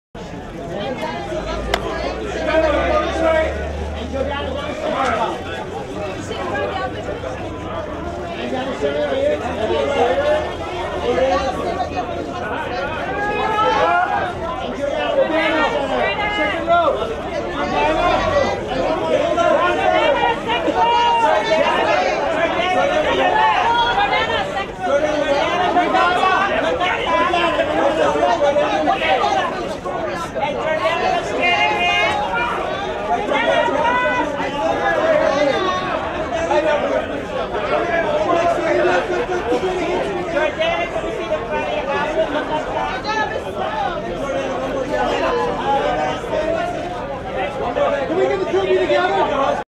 Jordana Brewster at A Quiet Place New York film Premiere
Jordana Brewster on the red carpet of A Quiet Place film Premiere at AMC Lincoln Square Theater in New York
New York City, NY, USA on Monday April 2, 2018